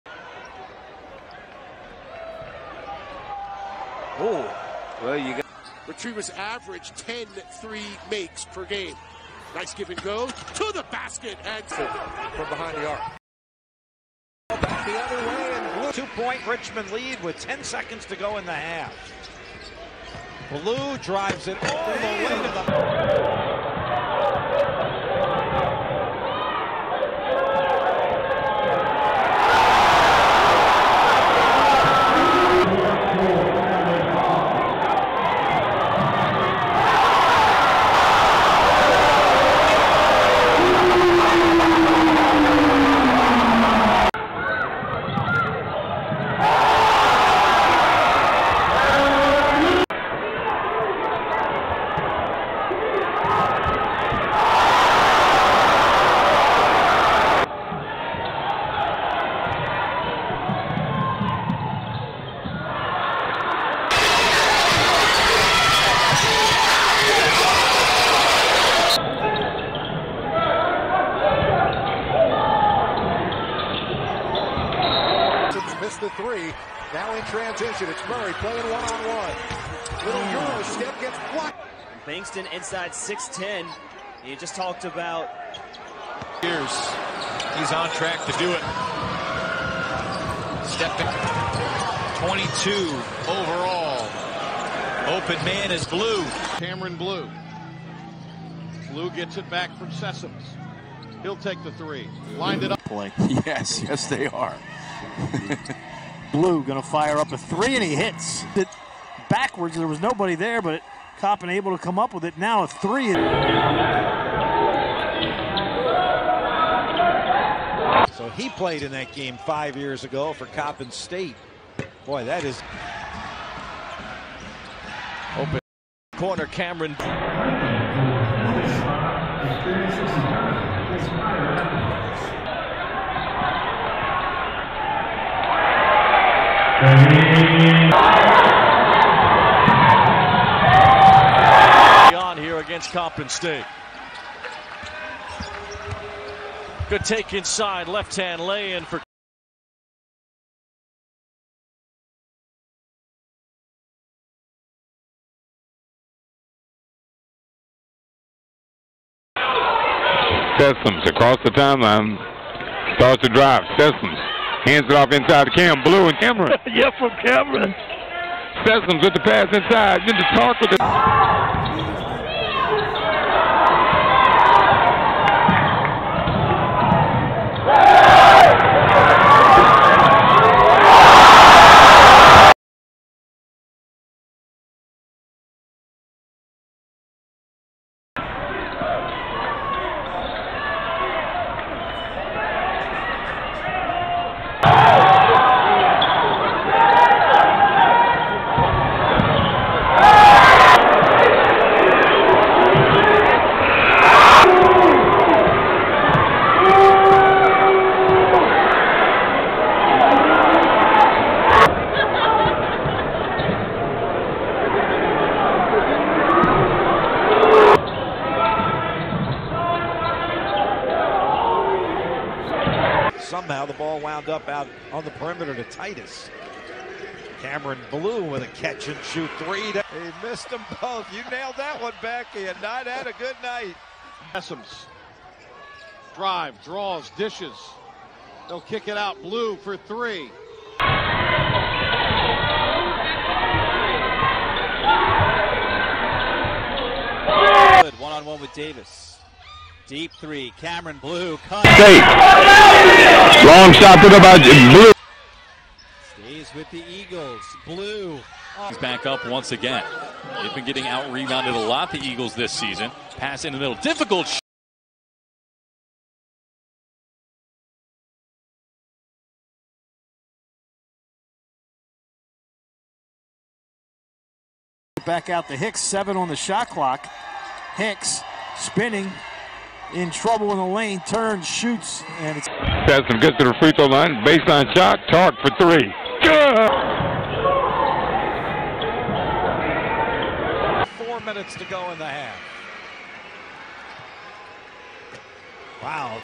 Kam'Ron Blue Coppin State Eagles 2022-2023 Highlights
Kam'Ron Blue meets a variety of important criteria on both offense and defense. At 6'7" and 205 pounds, he possesses a rare combination of athleticism and physicality, allowing him to guard both perimeter guards and mobile bigs with ease. Additionally, he has the ability to stretch the floor from beyond the NBA line, making him a versatile offensive threat. During his time at Coppin State, Kam'Ron Blue had a solid collegiate career, with an average of 6.6 points and 3.4 rebounds per game.At Mississippi Valley University Blue achieved a career-best performance, scoring 34 points in a game against Alabama State.